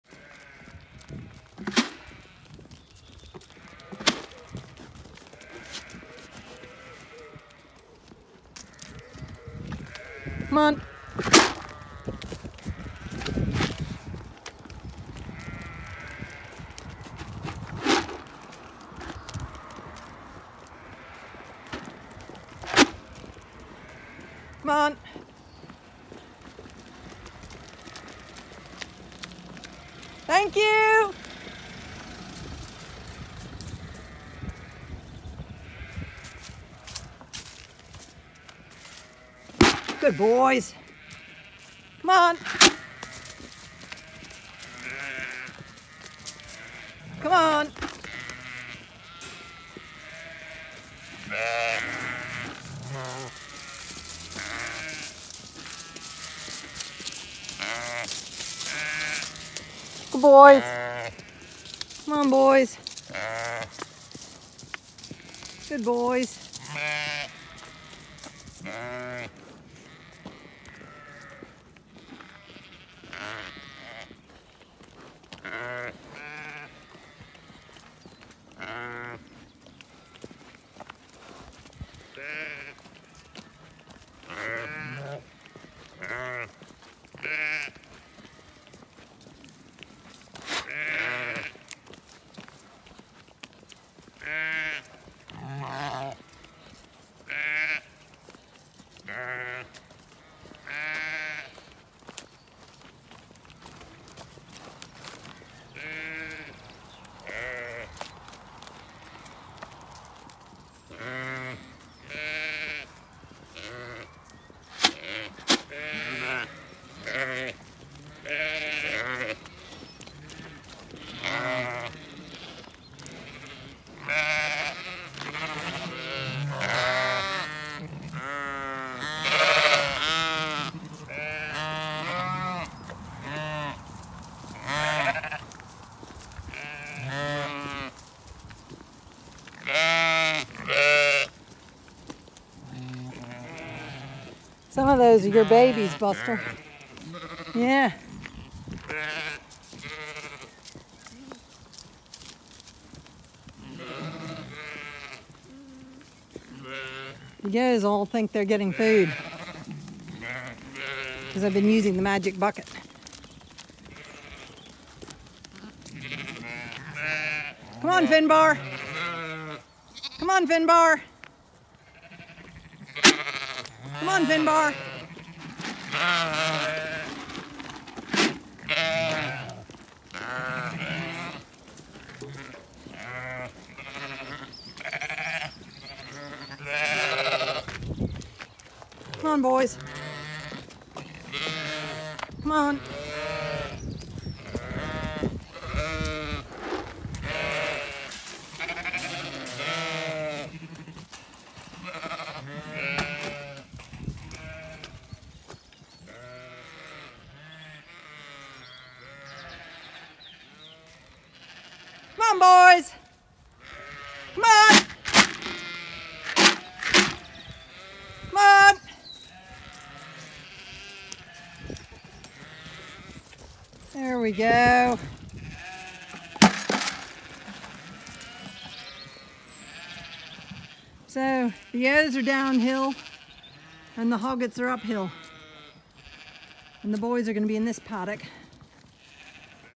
I moved the boys today. Normally hardly any traffic on a Sunday.
They were very happy to get some fresh grass. I really hope our weather stays kind for a this spring and no nasty surprises.